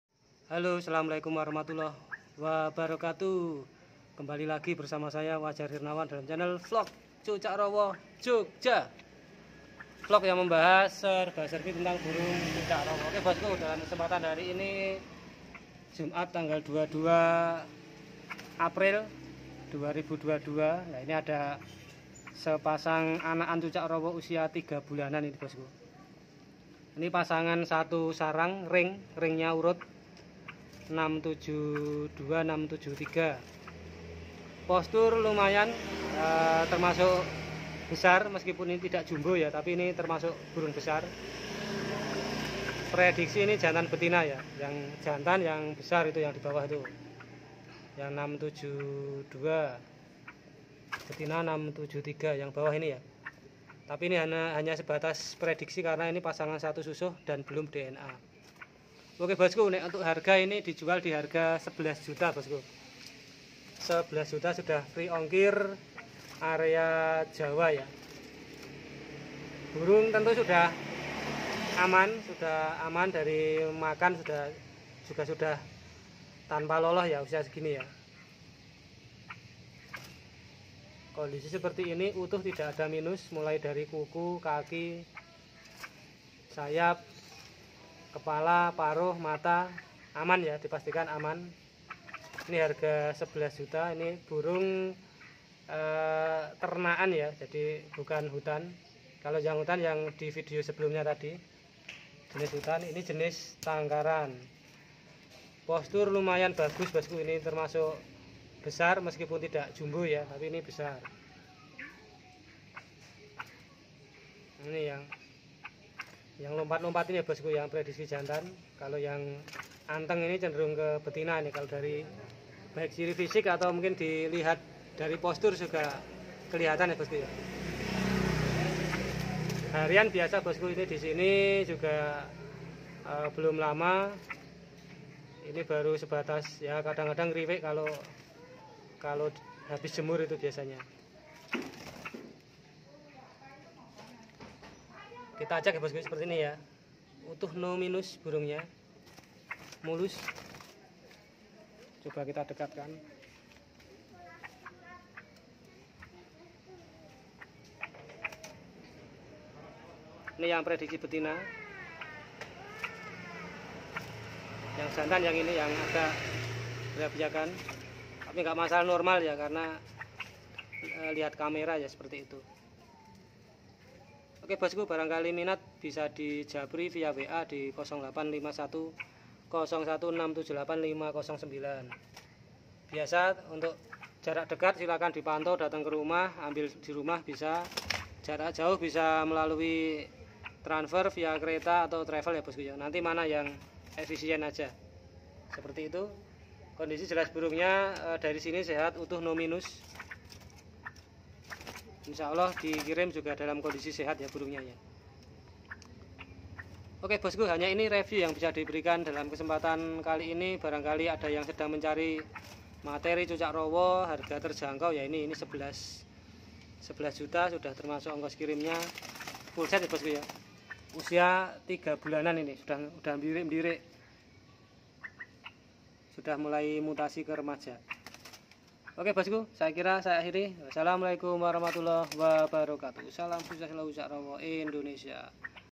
Review Anakan cucak rowo umur 3 bulan harga 11 juta Pasangan Sesusuh ring 672 dan 673
Ready sepasang sesusuh anakan cucak rowo umur 3 bulanan
kondisi mulus no minus
postur besar

Harga 11juta fulset

minat langsung aja WA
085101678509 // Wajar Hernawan